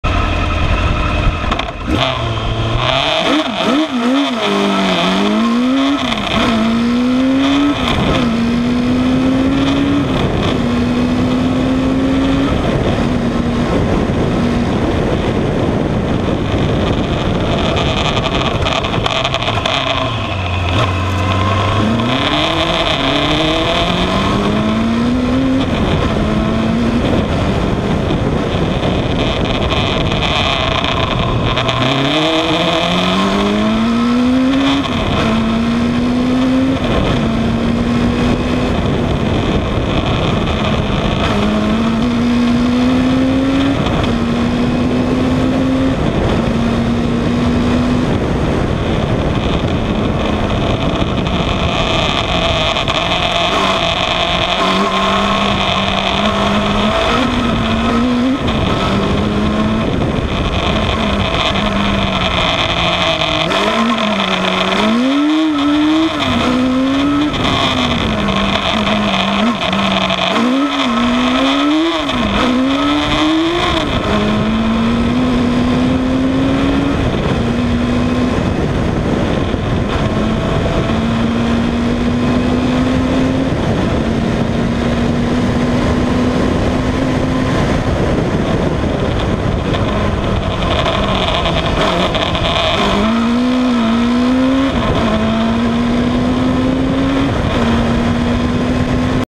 Debden Sprint, May 2016
Terrapin ZZR 1100 single-seater racing car
Driver: Simon Wallis